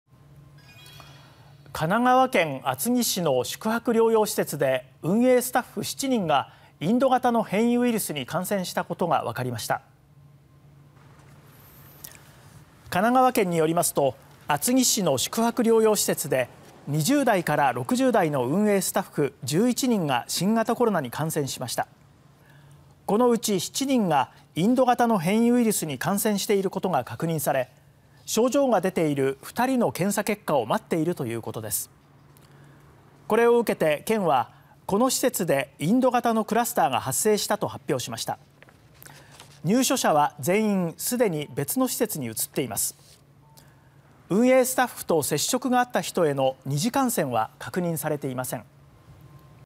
療養施設でインド型クラスター スタッフ7人感染(2021年6月4日)
神奈川県厚木市の宿泊療養施設で、運営スタッフ7人がインド型の変異ウイルスに感染したことが分かりました。

　神奈川県によりますと、厚木市の宿泊療養施設で20代から60代の運営スタッフ11人が新型コロナに感染しました。

　このうち7人がインド型の変異ウイルスに感染していることが確認され、症状が出ている2人の検査結果を待っているということです。

　これを受けて県は、この施設でインド型のクラスターが発生したと発表しました。

　入所者は全員、すでに別の施設に移っています。

　運営スタッフと接触があった人への二次感染は確認されていません。